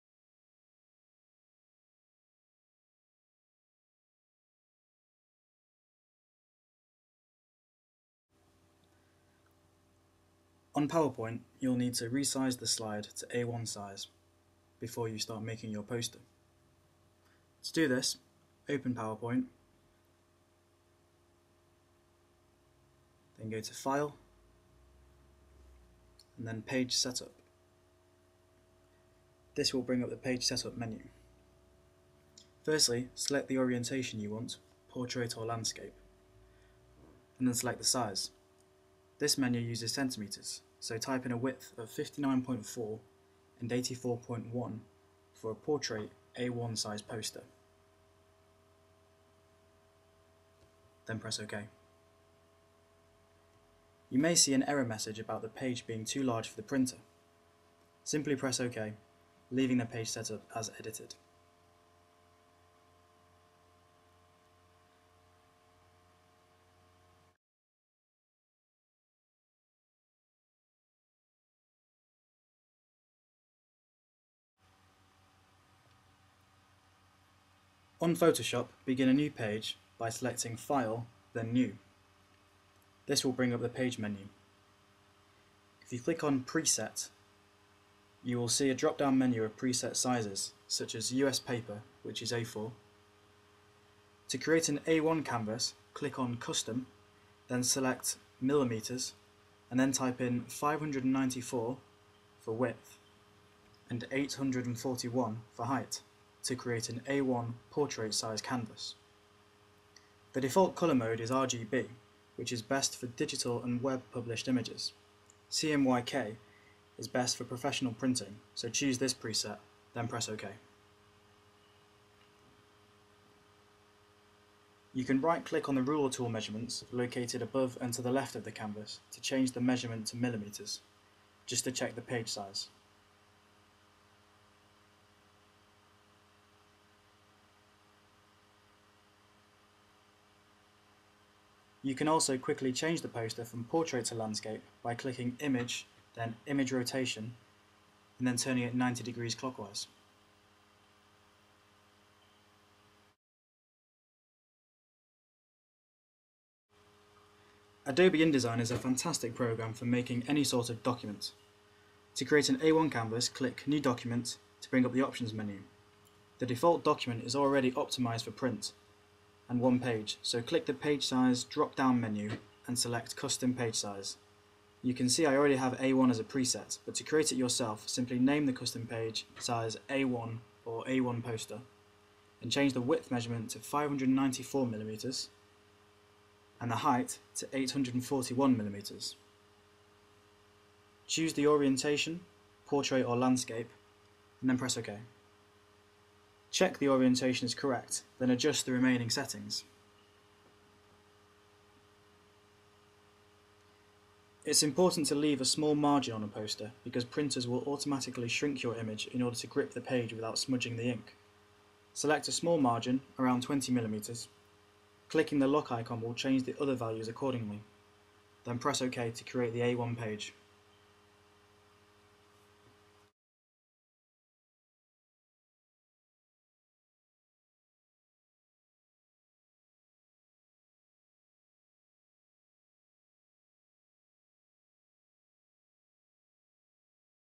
Tutorial: Creating an A1 Poster
A tutorial to show how to resize various design applications to an A1 size canvas. This video covers Microsoft PowerPoint, Adobe Photoshop and Adobe InDesgn. for more information.